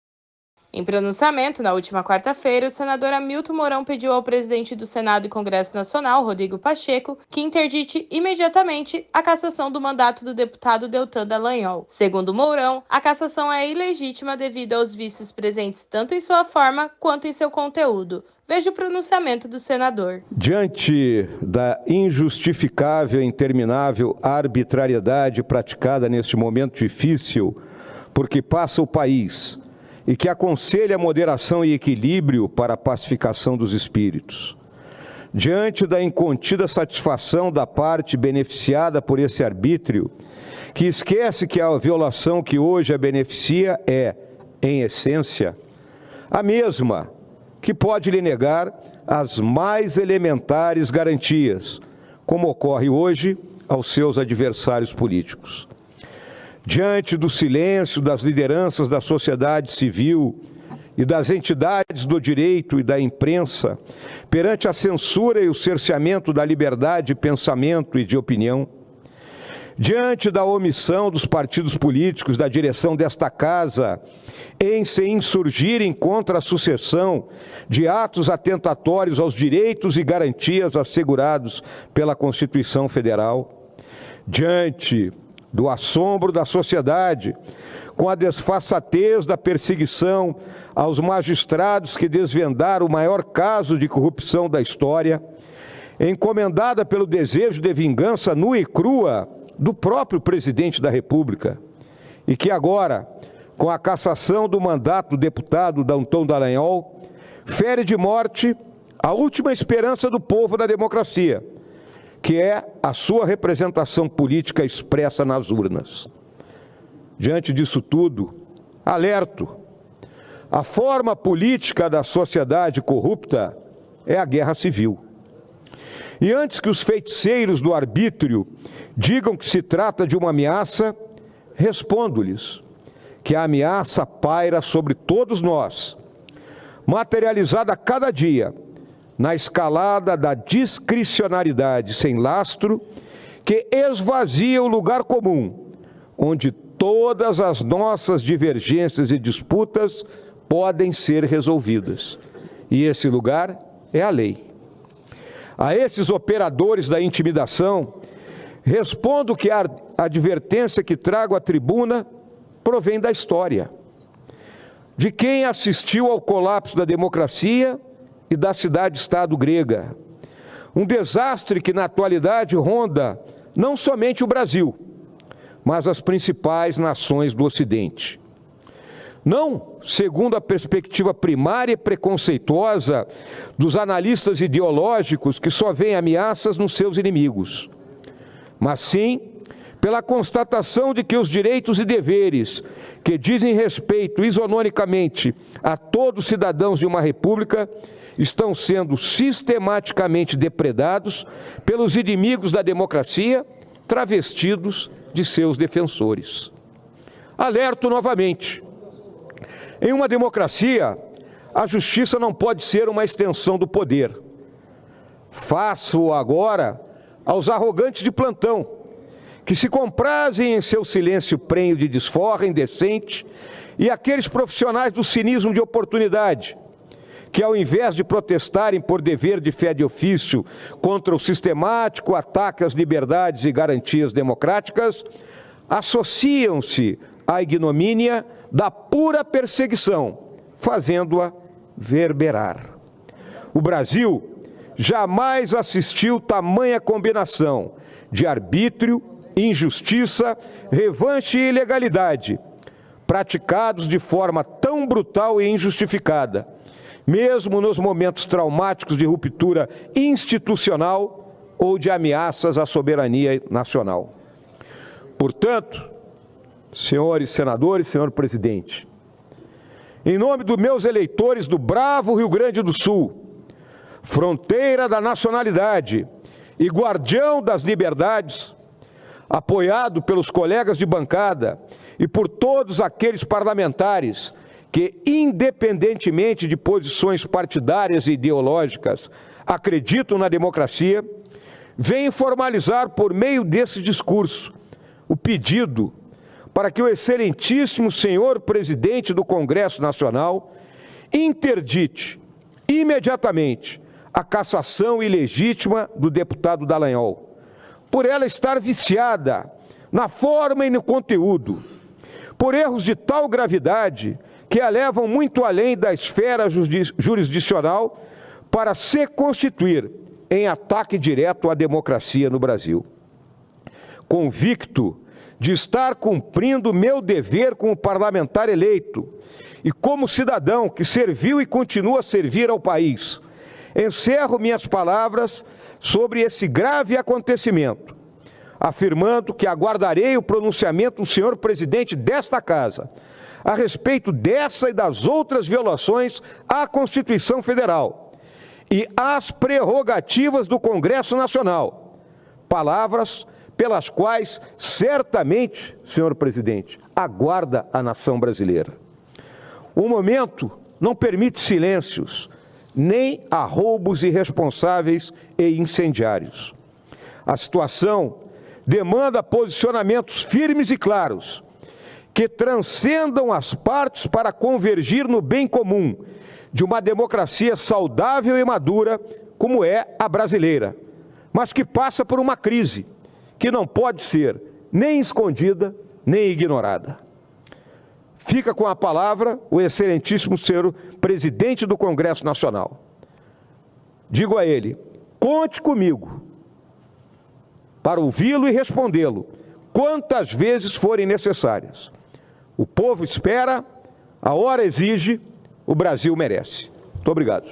Mourão pede "imediata interdição" de cassação de Deltan Dallagnol
Em pronunciamento na última quarta-feira, o senador Hamilton Mourão pediu ao presidente do Senado e Congresso Nacional, Rodrigo Pacheco, que “interdite imediatamente” a cassação do mandato do deputado Deltan Dallagnol.